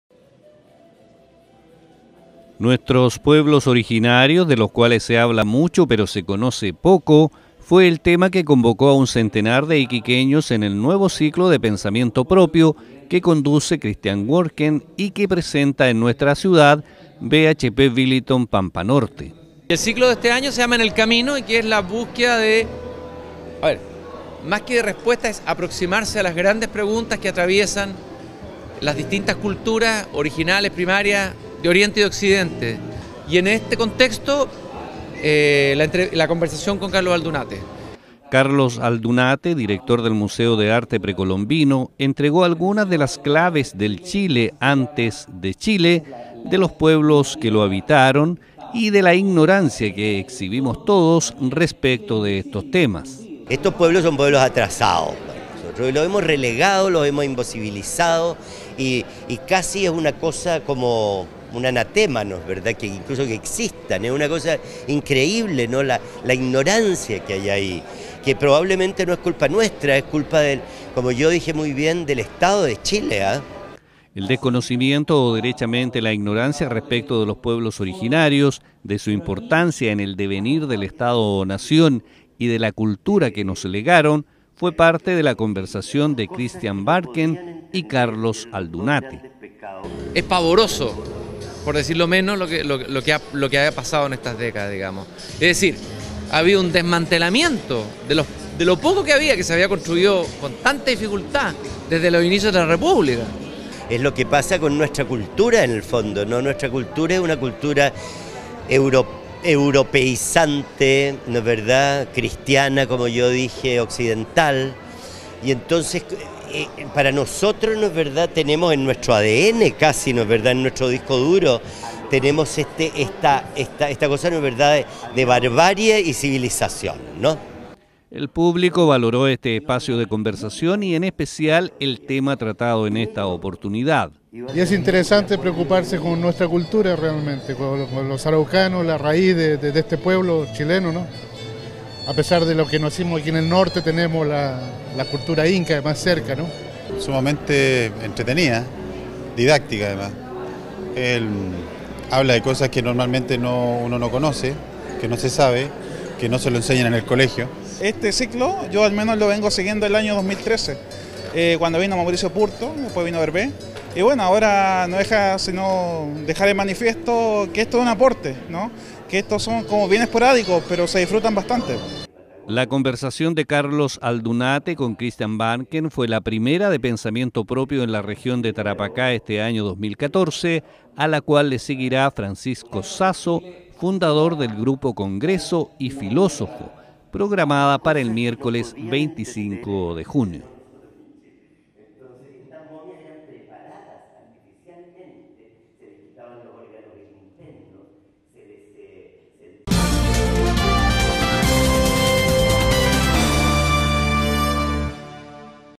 PARTIÓ NUEVO CICLO DE "PENSAMIENTO PROPIO" - Iquique TV
(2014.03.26) Con la presencia del arqueólogo Carlos Aldunate, director del Museo de Arte Precolombino, continuó el ciclo de conversaciones de Cristián Warnken en nuestra ciudad; siempre presentado por BHP Billiton Pampa Norte.